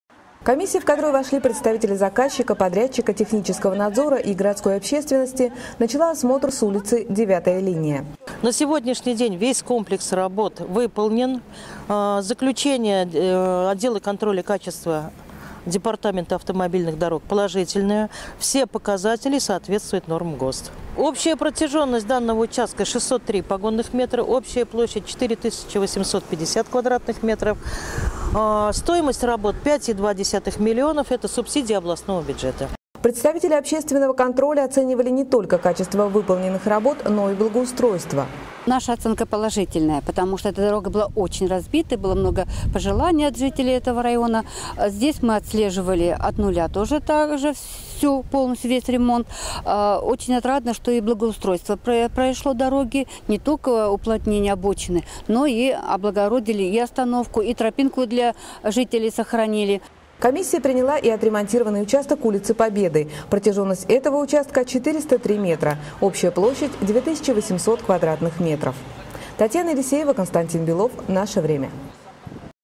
Качество ремонта городских дорог - на контроле у общественников
Межведомственная комиссия проверила отремонтированные дороги на улицах 9-й линии и Победы.